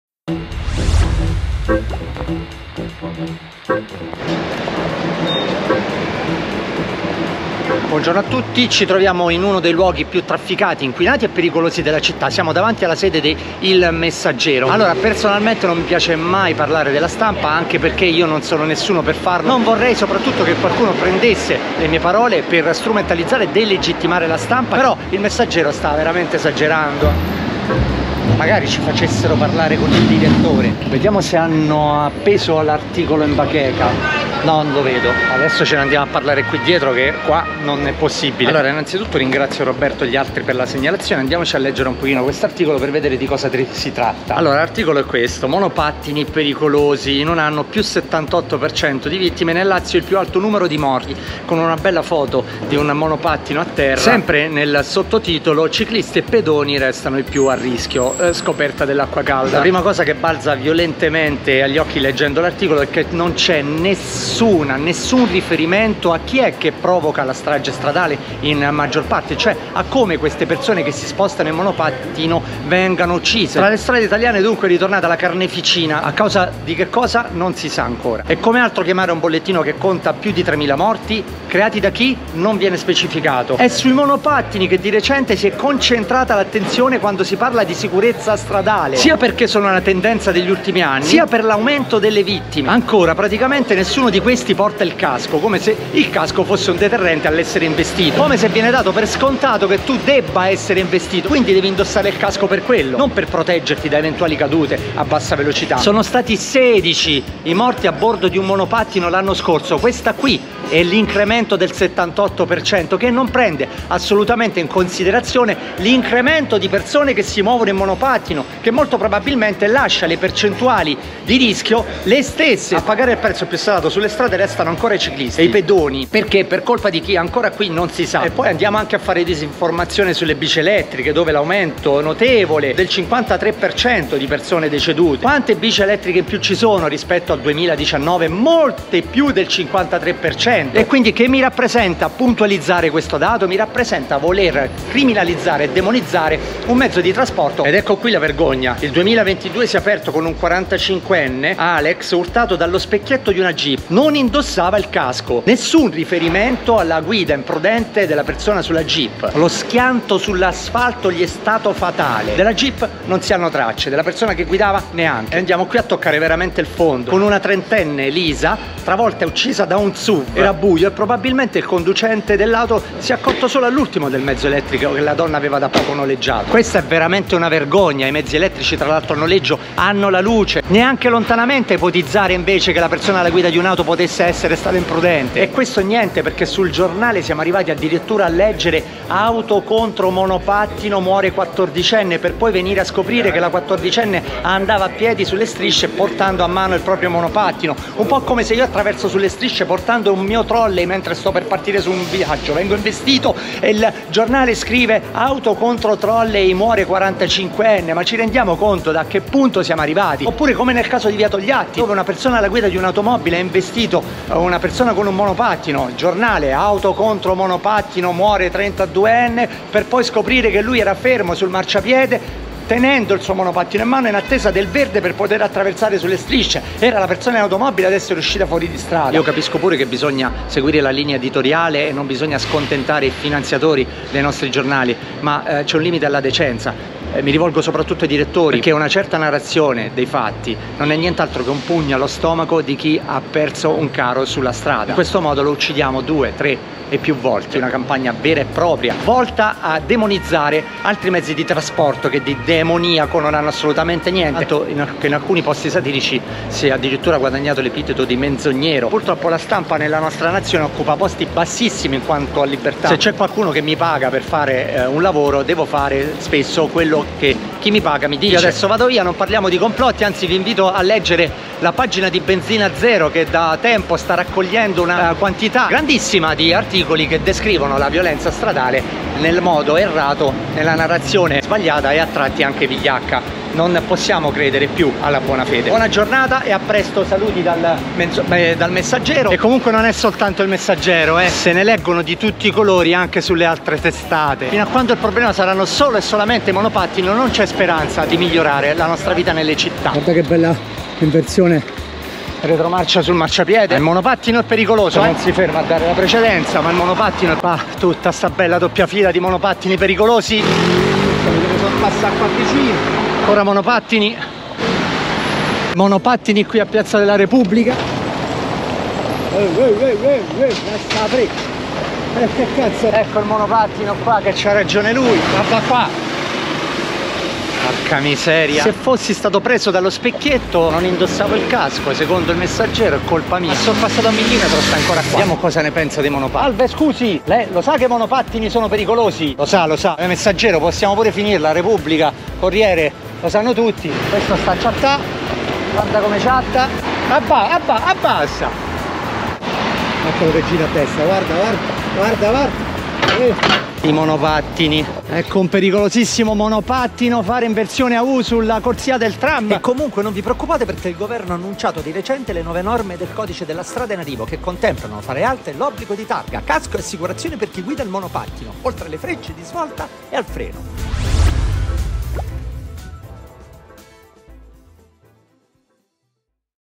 Come certa stampa racconta gli incidenti stradali
Sempre più spesso si discute di come la narrazione giornalistica incida sempre meno indirettamente sul fenomeno della violenza stradale.

Le “tecniche”, perché ci appare sempre più difficile credere alla buona fede, sono consolidate.

1. Vittima protagonista nel titolo e ed evidenza al mezzo guidato in modo da demonizzarlo.
2. Forma passiva nel titolo, non uomo uccide con la propria auto donna ma donna uccisa su monopattino.
3. Auto animata e guidata da nessuno nel titolo.
4. Eufemistico ‘impatto’ invece di scontro nel sottotitolo e nell’articolo
5. Giro di parole per non scrivere tamponamento o investimento nell’articolo  usando terminologia tipo “urtato da”
6. Nessun cenno all’automobilista alla guida dell’auto investitrice, che risulta invisibile o inesistente.
7. Ricerca di attenuanti a deresponsabilizzare autista come “forse la scarsa luminosità”
8. Colpevolizzazione della vittima con dettagli inutili, come investita “di notte”, finita sotto il camion, non aveva il casco.
9. Etc

Ci siamo abituati ma ultimamente si comincia davvero a superare il limite della decenza.
Come fa il Messaggero in un articolo, dove si evidenzia un dato senza citare in alcun modo il responsabile. Non è la sola testata per carità, ma qui a Roma si comincia ad arrivare al limite della decenza.
Più che impreparati iniziamo a sembrare dei vigliacchi.

L' Italia occupa un posto molto basso nella classifica per la libertà di stampa, capiamo che bisogna seguire la linea editoriale e non scontentare gli sponsor ma ogni articolo siffatto non è altro che un pugno nello stomaco di chi sopravvive alla vittima “che sente morire la propria cara nuovamente ed inutilmente”.